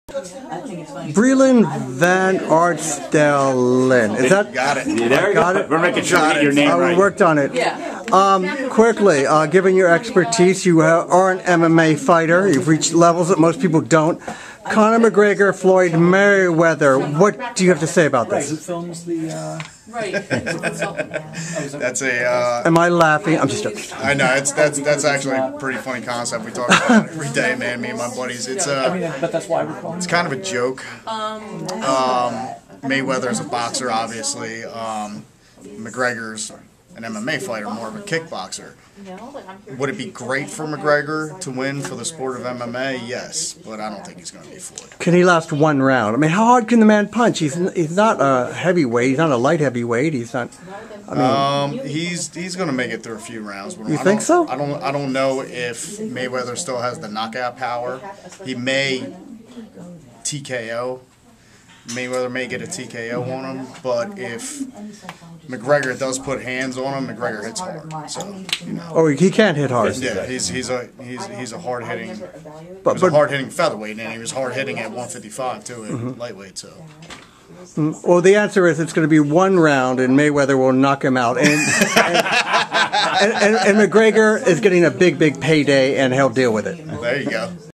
Van Artsdalen in the BWB greenroom
The MMA adrenaline junkie, gives his take on the upcoming,
Floyd Mayweather Jr. vs. Conor McGregor fight.
Followed by Adam's declarative prediction!